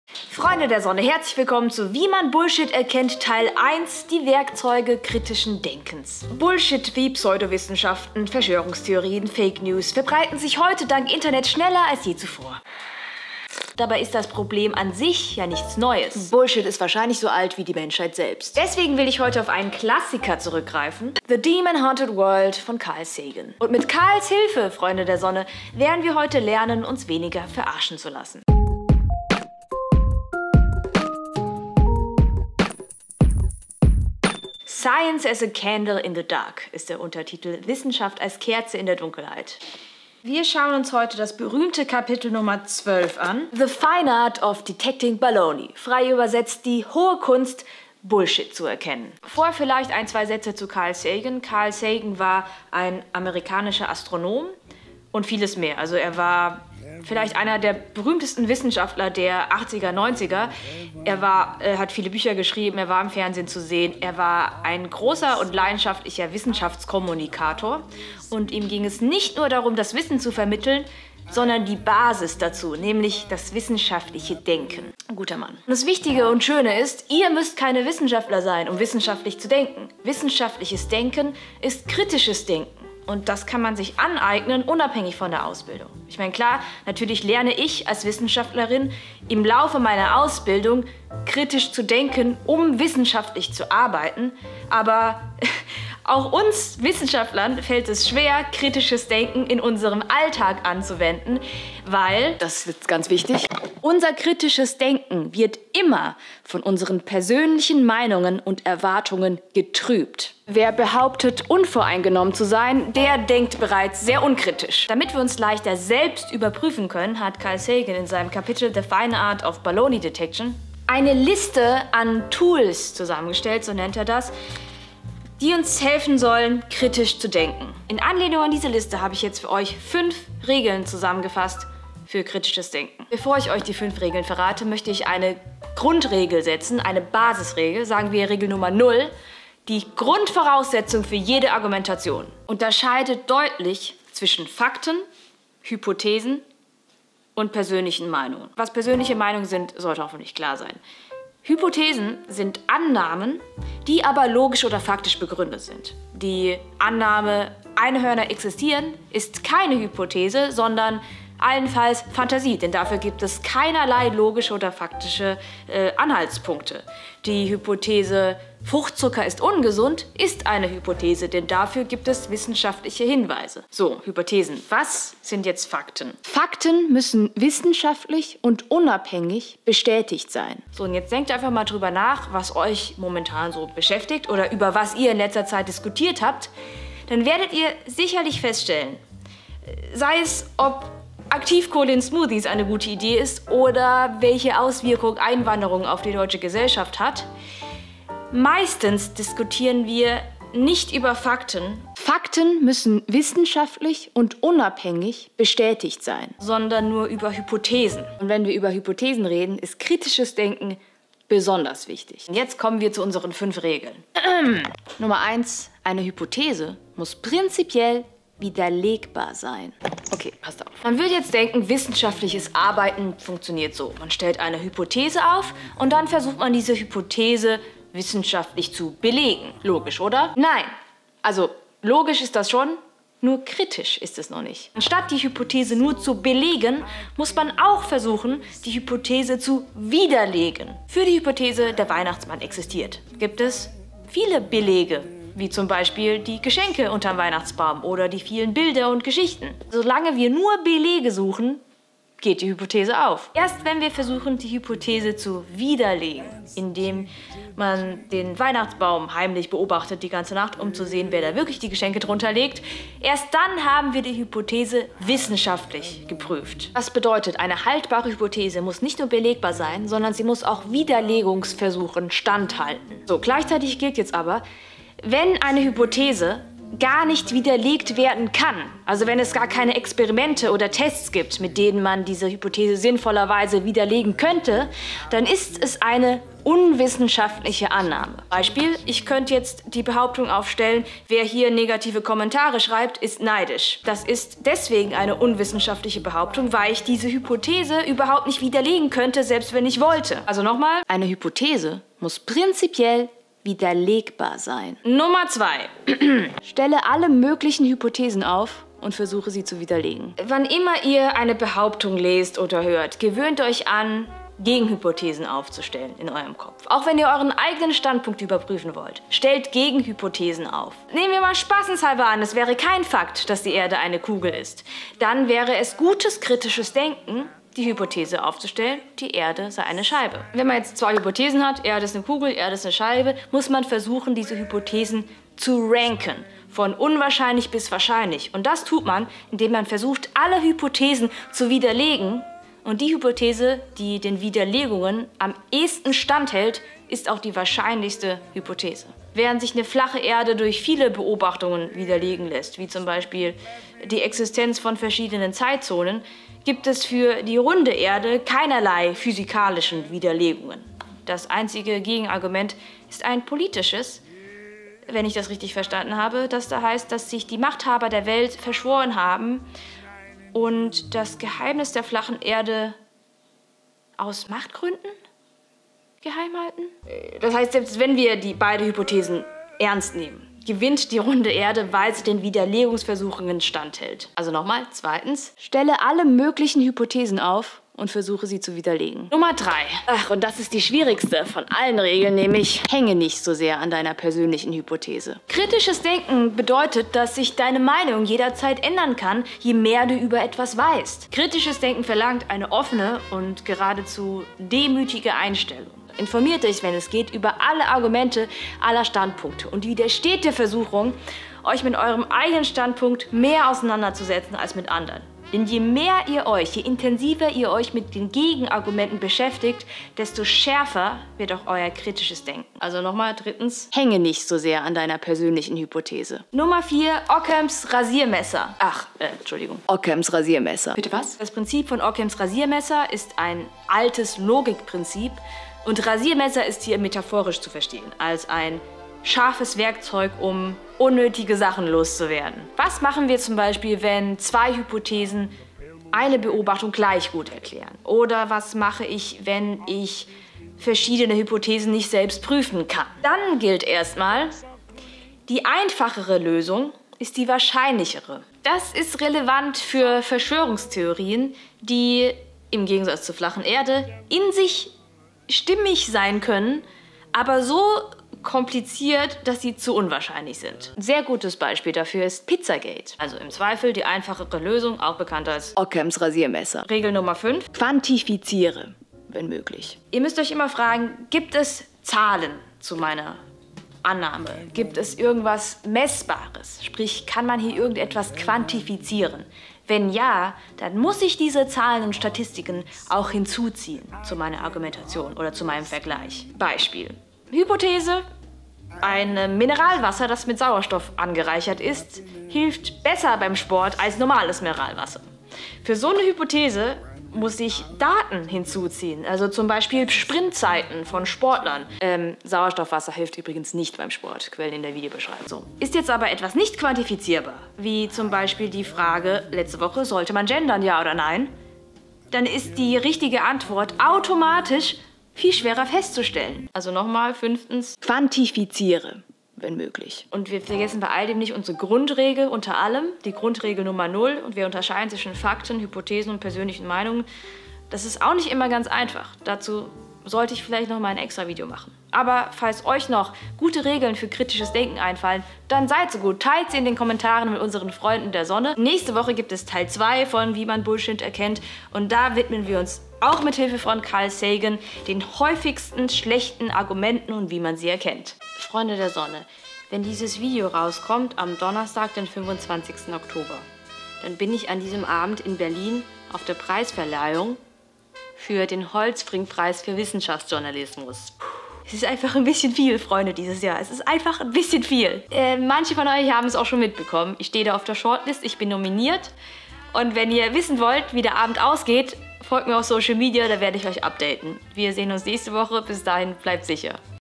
Die Kunst Bullshit zu erkennen
Pseudowissenschaft, Verschwörungstheorien und Fake News - Wie man Bullshit erkennt nach Carl Sagan’s Klassiker “The Fine Art of Baloney Detection”

Quelle
Sagan, Carl. The demon-haunted world: Science as a candle in the dark. Ballantine Books, 2011.